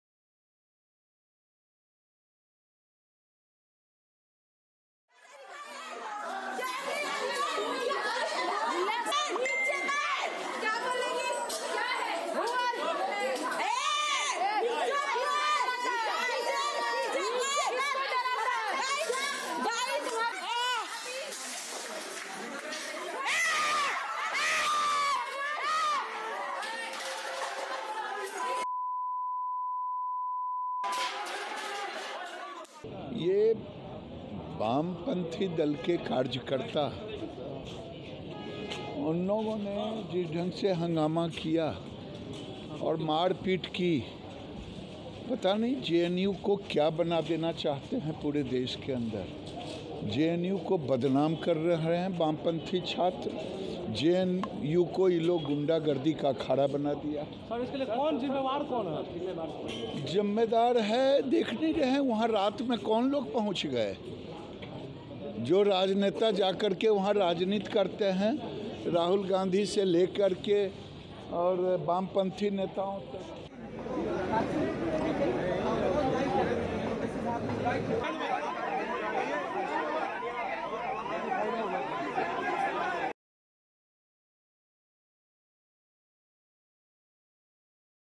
Giriraj Singh blames left party workers for JNU violence
Patna (Bihar), Jan 06 (ANI): While speaking to ANI on JNU violence, where students including professors received injuries after they were assaulted by masked goons, Animal Husbandry Minister Giriraj Singh blamed left party workers for violence in JNU.